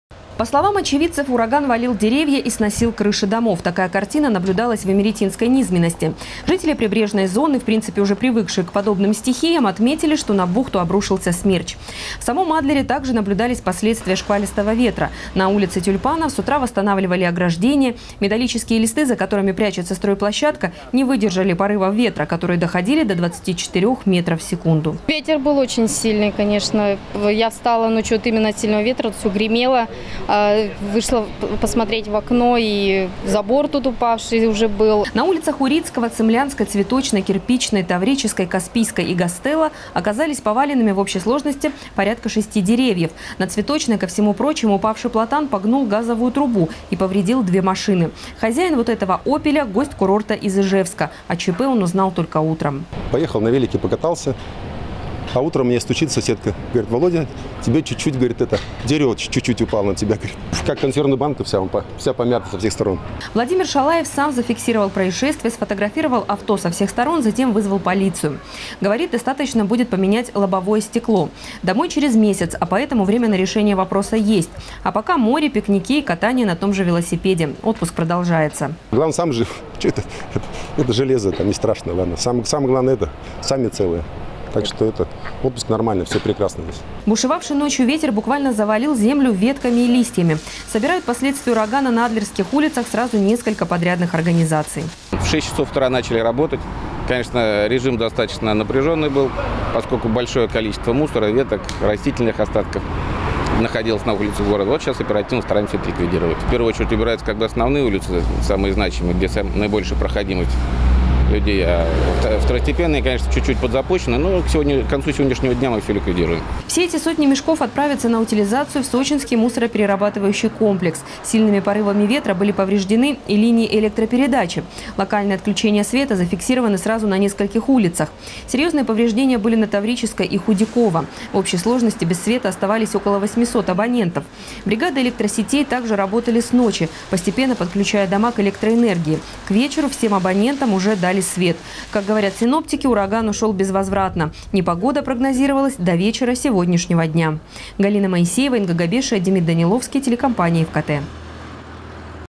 На Сочи этой ночью обрушился ураган. Новости Сочи Эфкате
Сильный порывистый ветер налетел в основном на Адлерский район курорта. Штормовое предупреждение было объявлено ещё в начале недели. Начавшись в полночь, ураган бушевал несколько часов. С ночи и весь сегодняшний день, на пострадавших улицах трудились и сотрудники администрации, а рабочие всех коммунальных служб: в том числе МЧС и бригады электросетей.
Сочи. Новости.
Телеканал Эфкате.